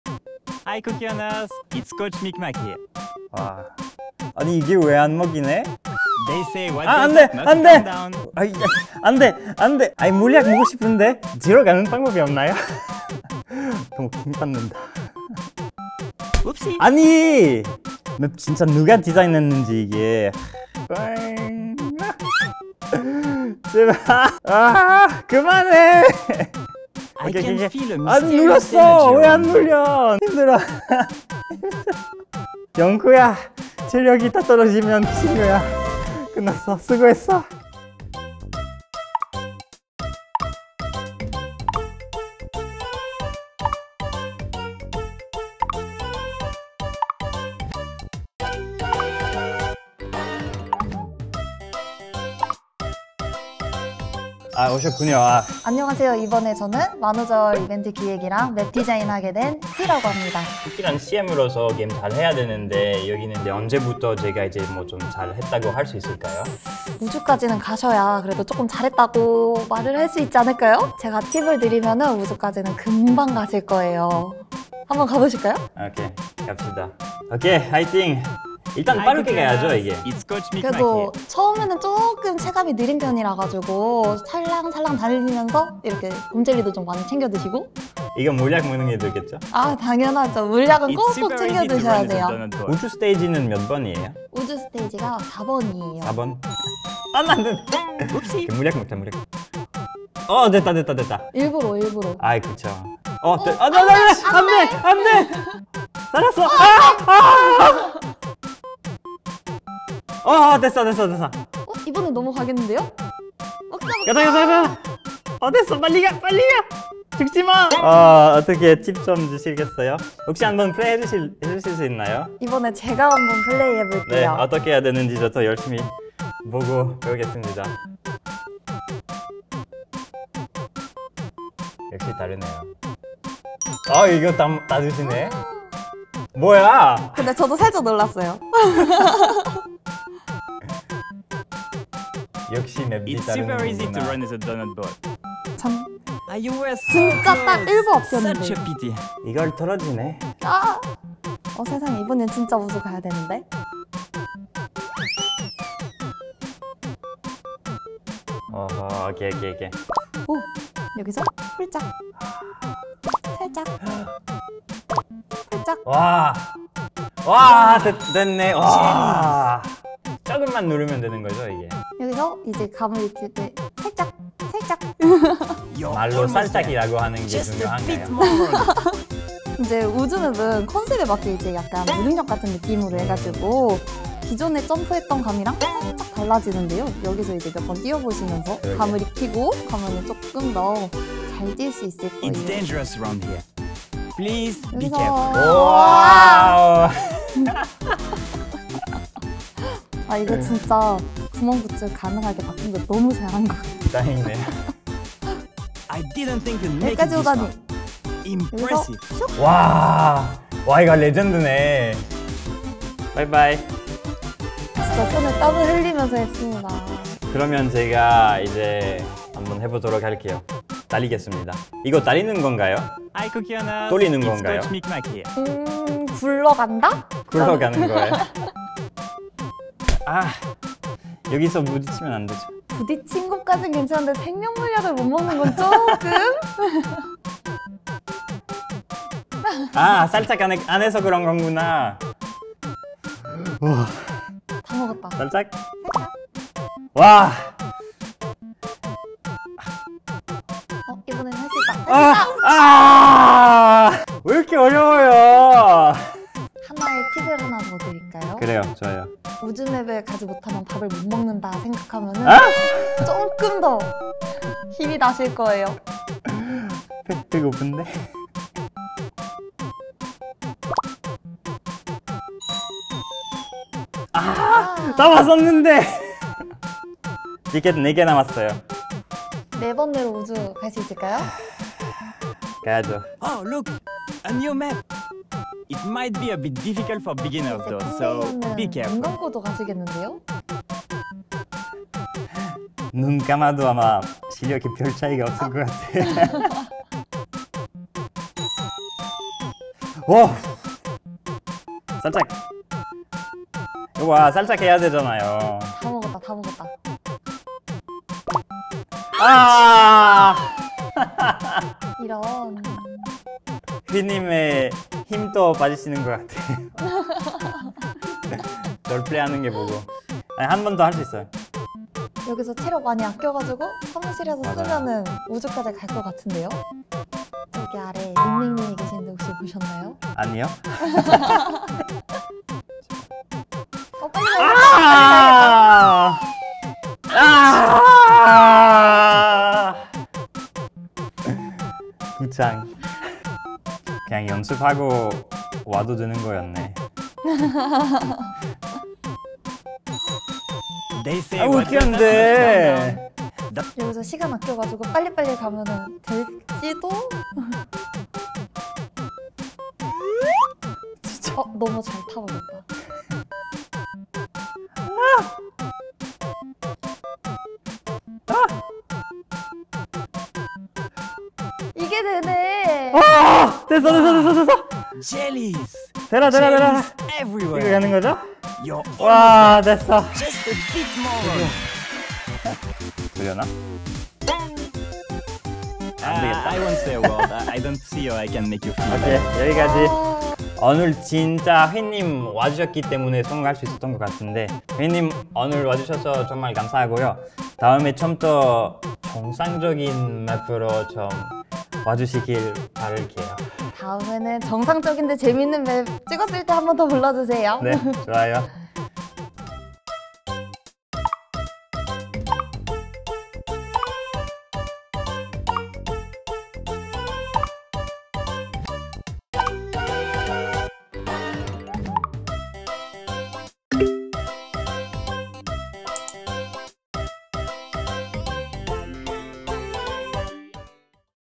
[믹맥의 도전] 도~넛
만우절 모드를 처음 플레이 해본 CM 믹맥.
"이거 만든 사람 나와아아앍!!"